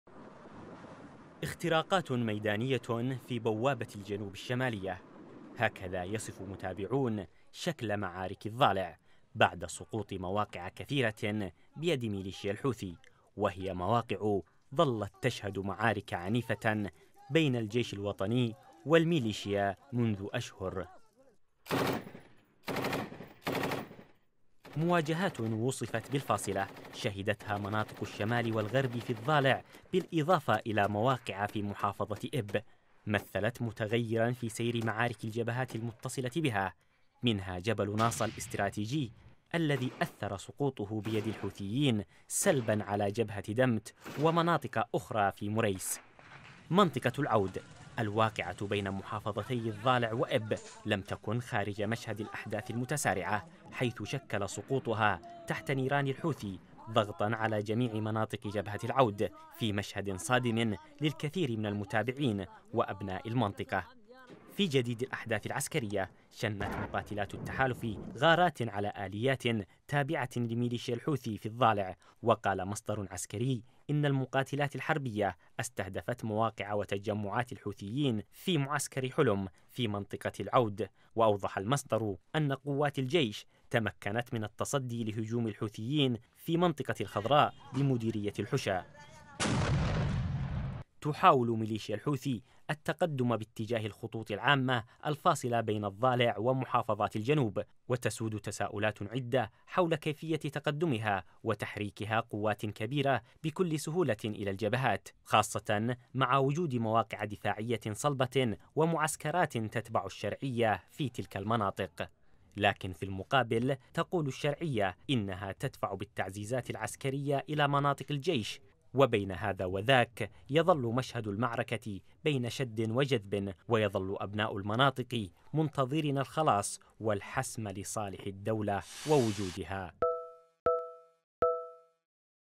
سقوط مواقع في الضالع بيد الحوثي.. اهمال حكومي ام اختراقات ميدانية؟ | تقرير: محمد عبدالمغني
تدور معارك عنيفة بين الجيش الوطني ومليشيا الحوثي في مواقع عدة بمحافظة الضالع .
وقالت مصادر عسكرية إن المعارك في جبهة عزاب بيت الشرحي أسفرت عن مقتل عدد من عناصر المليشيا، فيما قصفت قوات الجيش مواقع للحوثيين في منطقة العود غرب قعطبة...